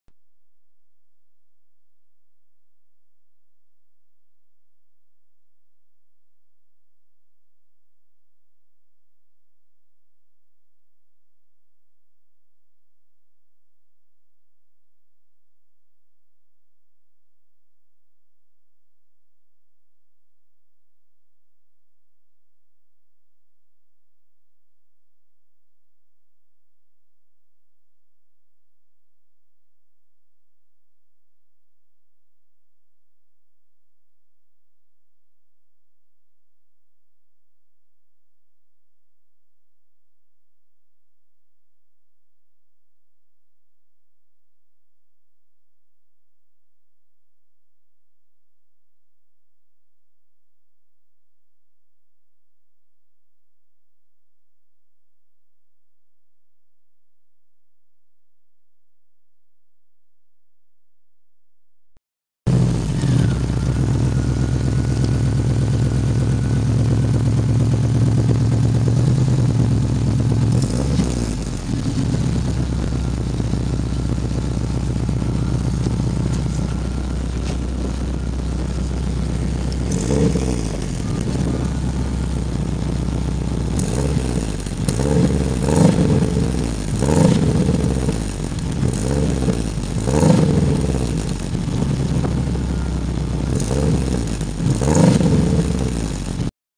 доработали звук глушителя 2110 ( звук в конце )
несколько месяцев назад ставили паук на 2110 , продавцы заверили , что после замены паука  звук выхлопа изменится и остальные глушители менять не нужно , но владелец остался недоволен звуком .
после ставились различные эксперементы , но звук устроил лишь тогда , когда вместо основного глушителя установили ещё одну стандартную среднюю часть от ваз 2110 и получили тот самый звук напоминающий рык SUBARU ...
сварочный аппарат , болгарка , старый глушитель , герметик выхлопной системы , подмер на месте .
Да, про шумку багажника забывать нельзя , автомобиль зашумован .